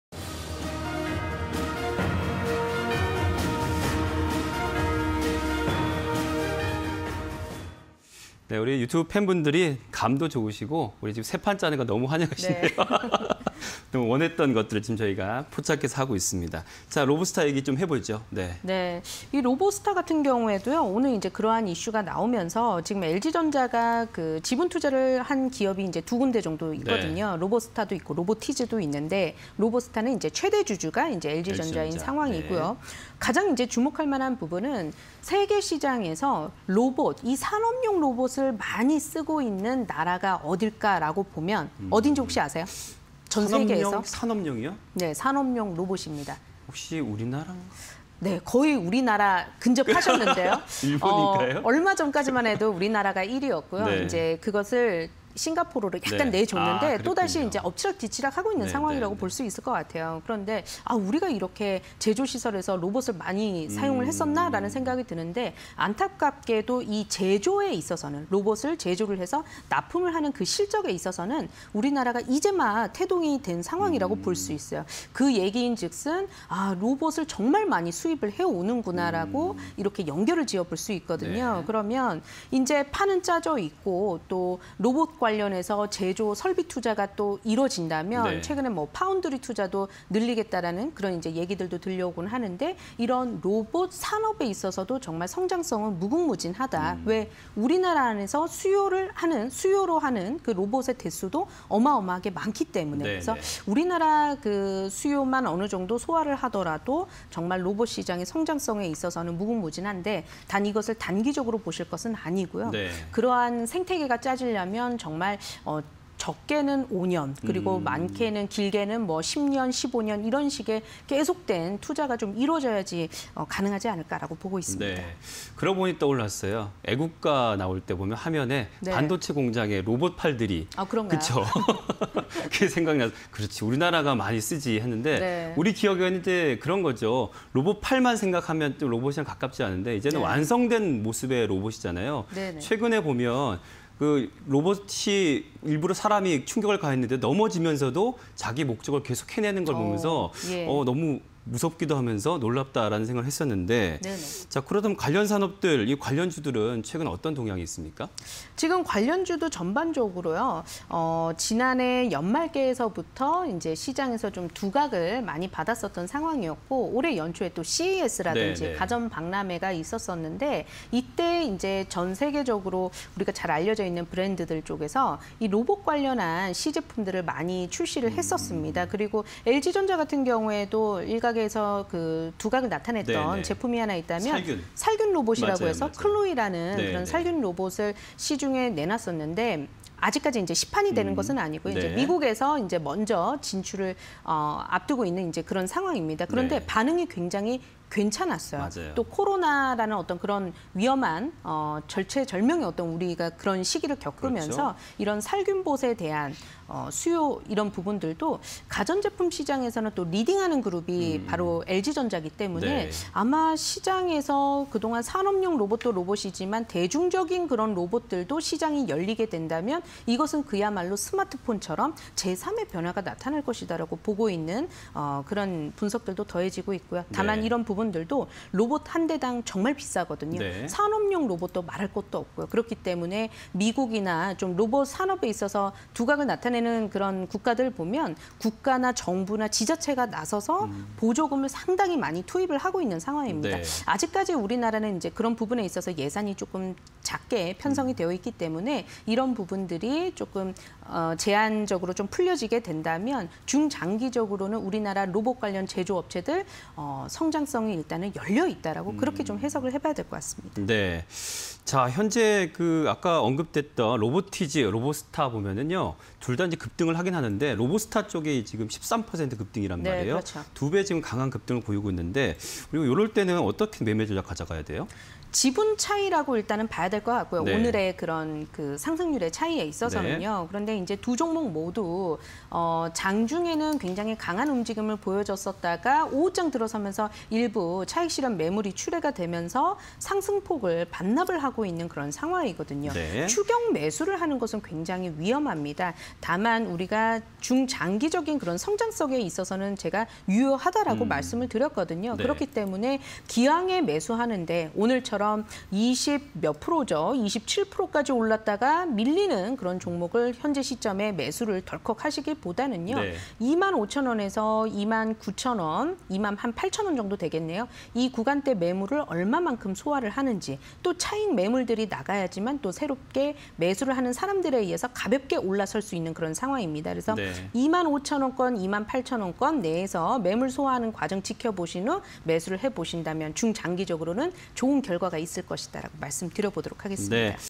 로보스타(090360)... LG전자, MC사업 철수… 로봇 부문 주력 기대_테마IN이슈 (20210405)
로보스타(090360)
- 제조용 로봇 제조 사업 영위
- LG전자, MC사업 철수… 로봇 부문 주력 기대